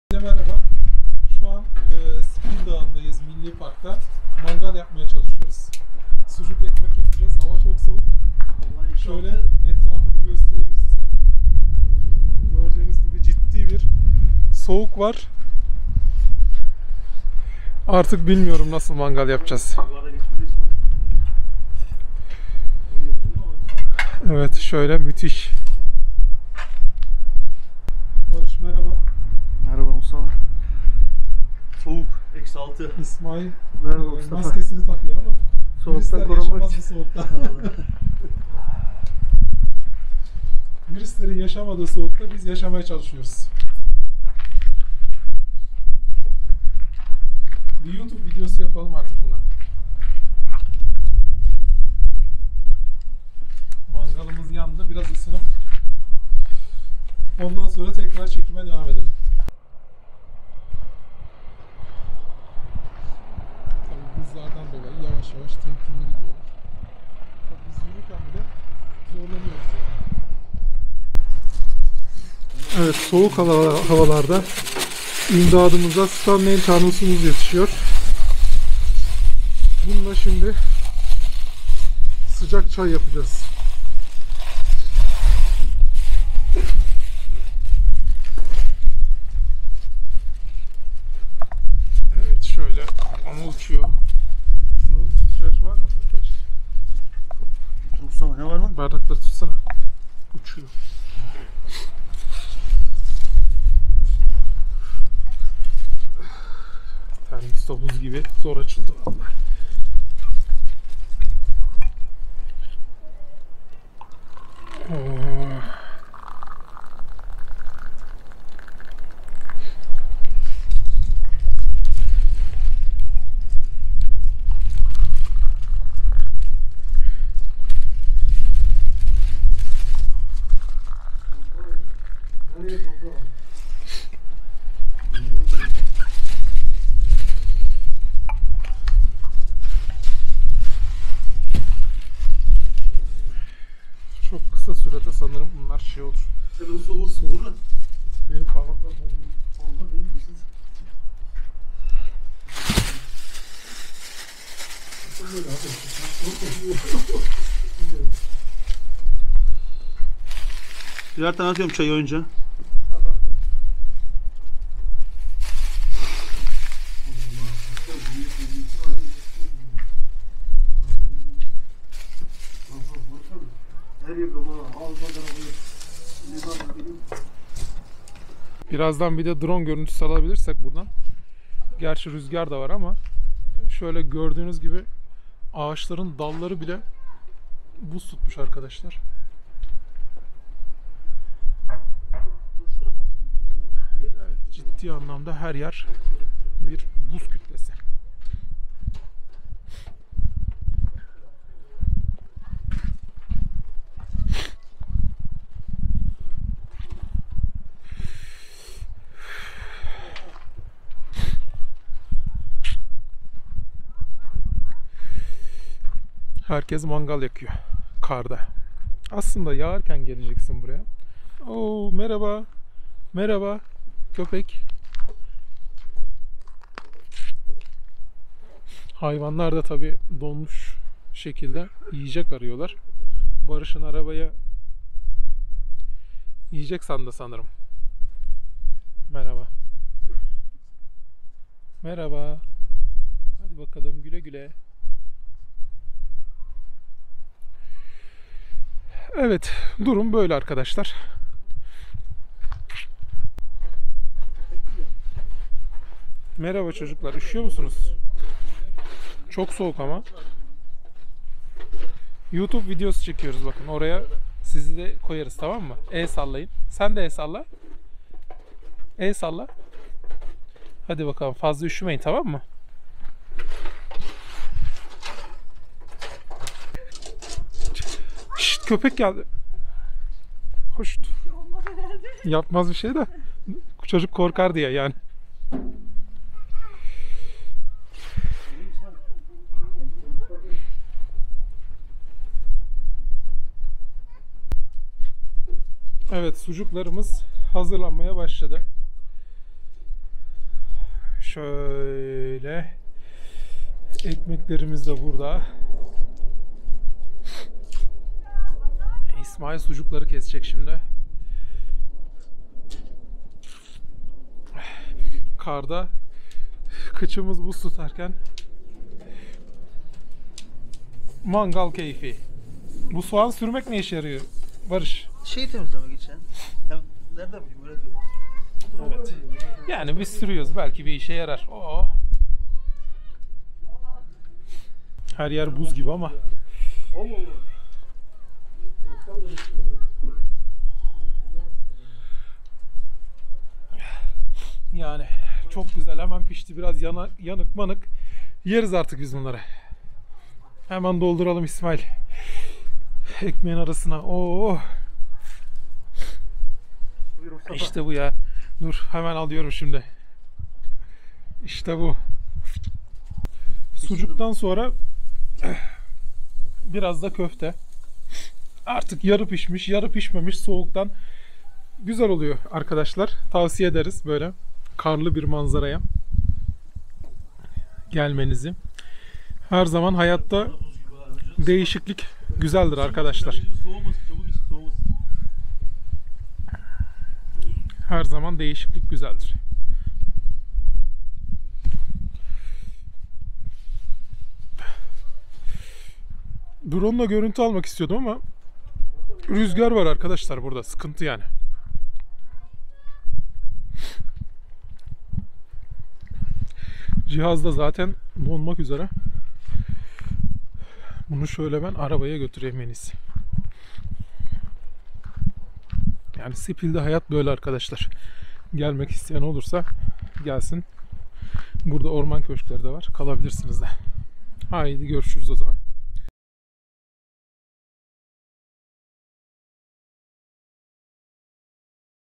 SPİL DAĞI BUZLARIN ARASINDA MANGAL!
Spil Dağı Milli Parkında buzların arasında mangal yaktık, dondurucu soğuk -6°C'yi gösterirken zor şartlarda mangal macerası. vlog